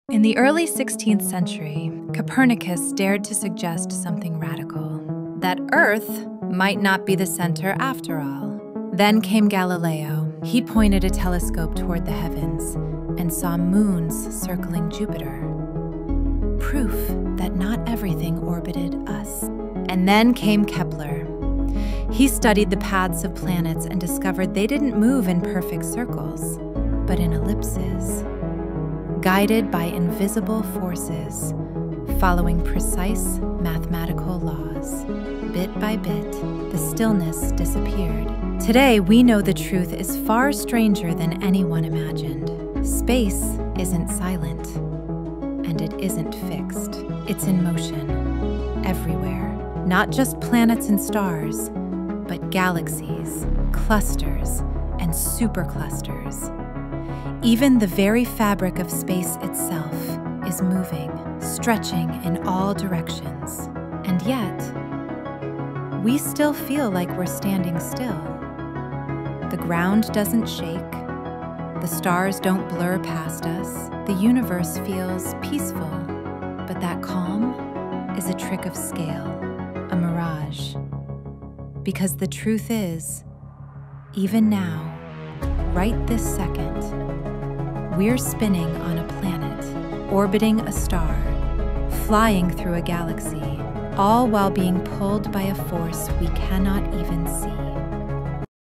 Early space exploration. 🪐🌌
For thousands of years, humanity believed the universe was still — silent, orderly, and built around us.
The stars seemed fixed. The heavens eternal.

But that calm was an illusion.

From Copernicus challenging Earth’s place, to Galileo witnessing moons orbit another world, to Kepler uncovering the hidden geometry of the cosmos, our understanding shattered piece by piece.

Today, we know the truth is far stranger.
Space is not quiet.
It is not motionless.

Everything is moving — planets, stars, galaxies, even space itself, stretching in all directions. And yet… we feel perfectly still.

This is the story of a universe in motion — and the illusion that hides it.